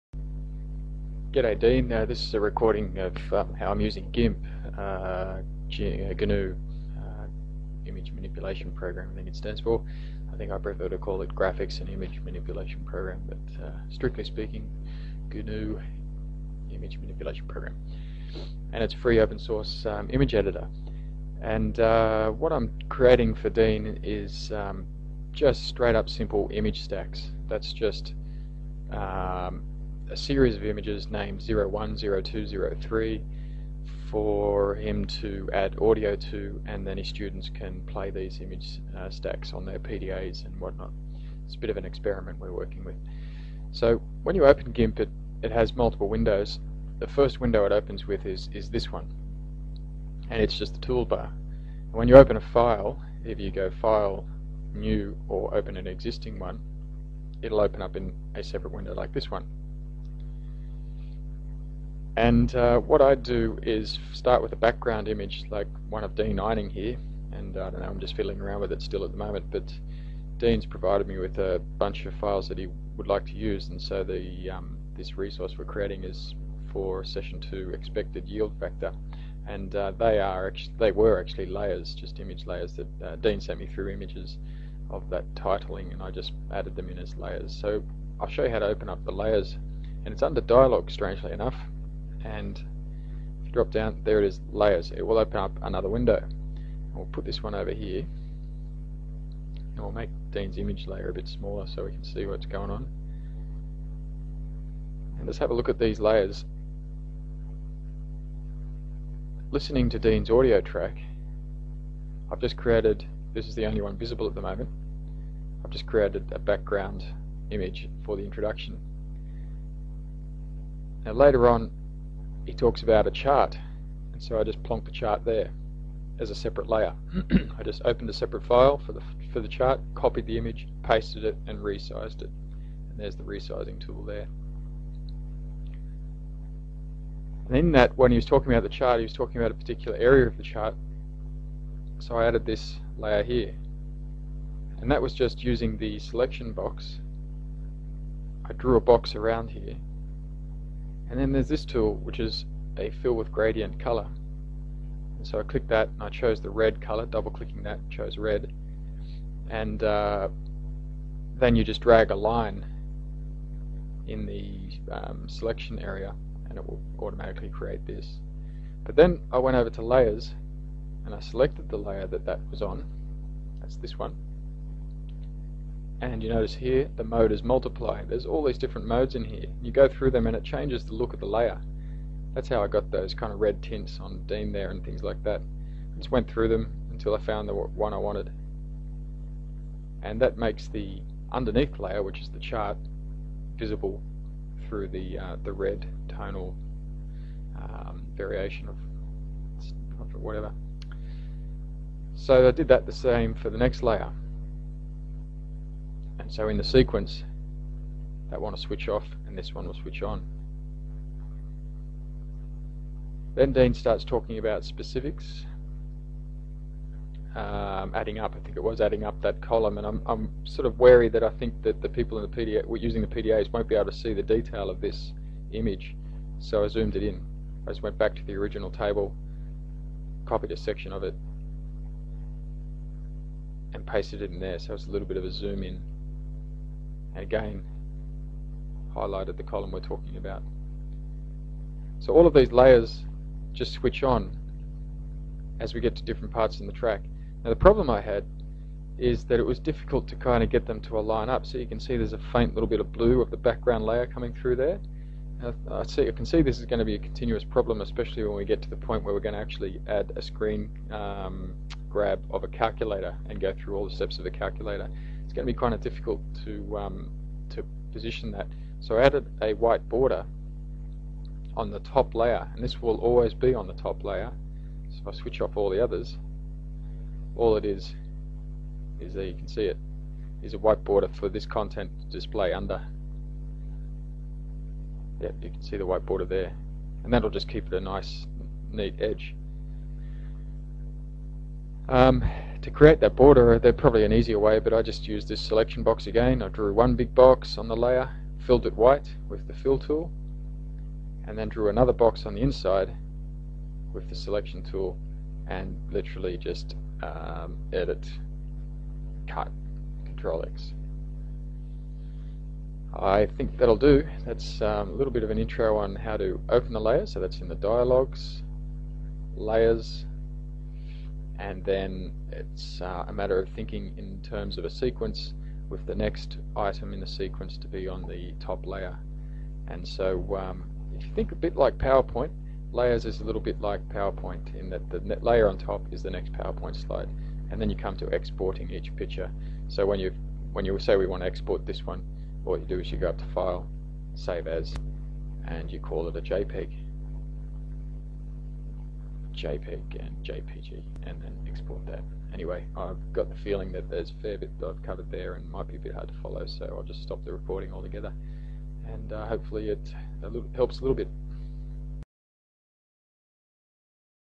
Gimp for image stacking
A demo of how to use GIMP to create image stacks